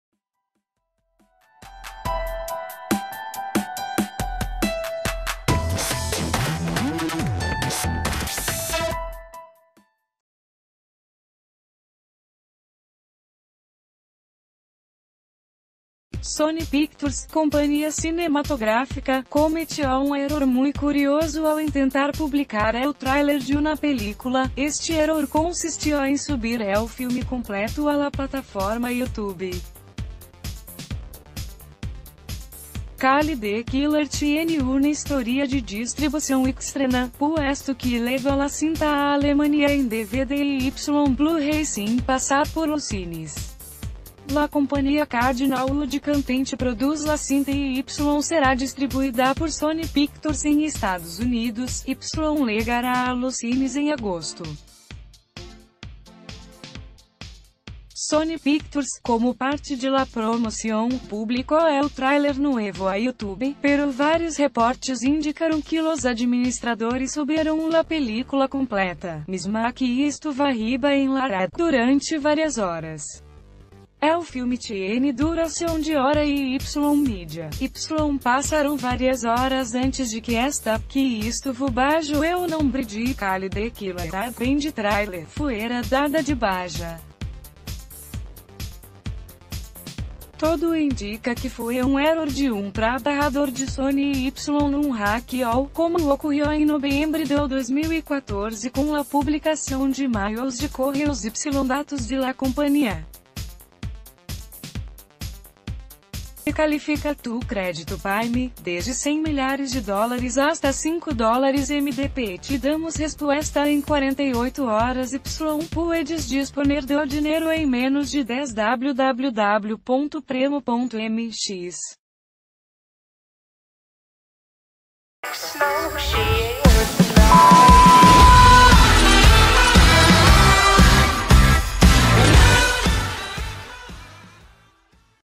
En vez de tráiler ¡suben la película completa!
Sony Pictures, compañía cinematográfica, cometió un error muy curioso al intentar publicar el tráiler de una película, este error consistió en subir el filme completo a la plataforma YouTube. Khali The Killer tiene una historia de distribución extraña, puesto que llegó la cinta a Alemania en DVD y B...